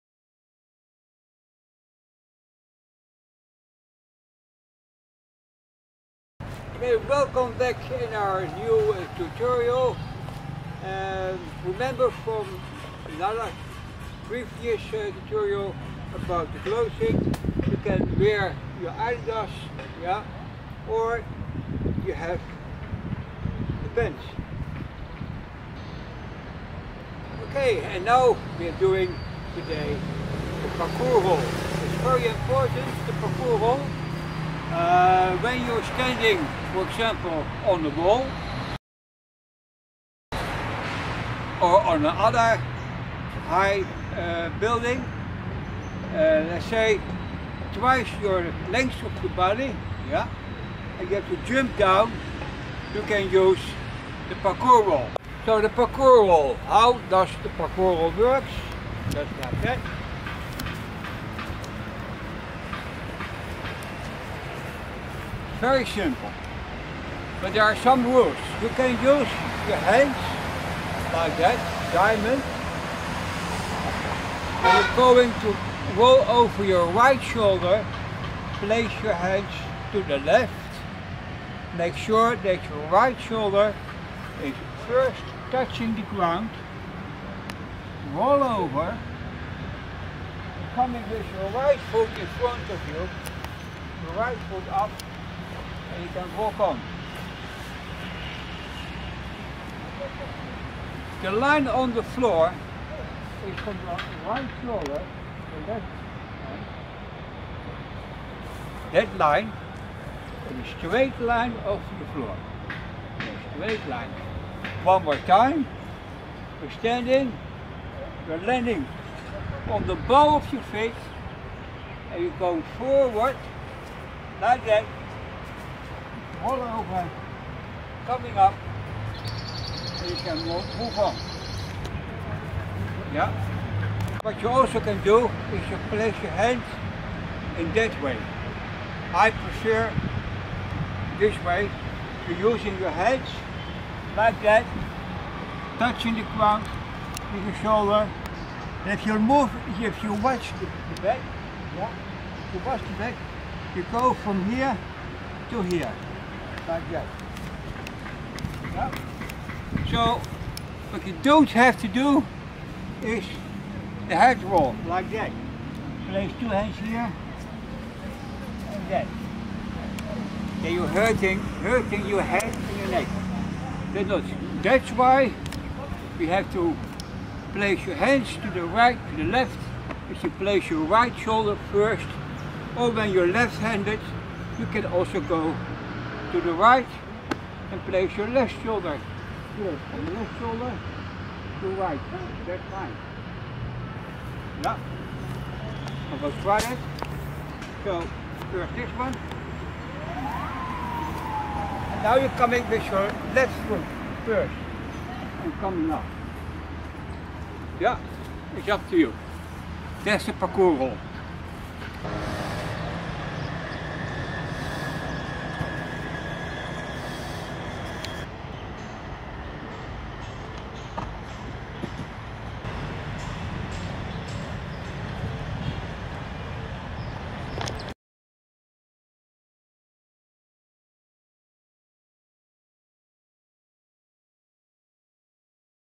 Tutorial 5 The Parkourrol
In this video you see the tutorial the parkourrol. Use it when you jump down from a certain height (starting 1,5 to twice your body lenghth)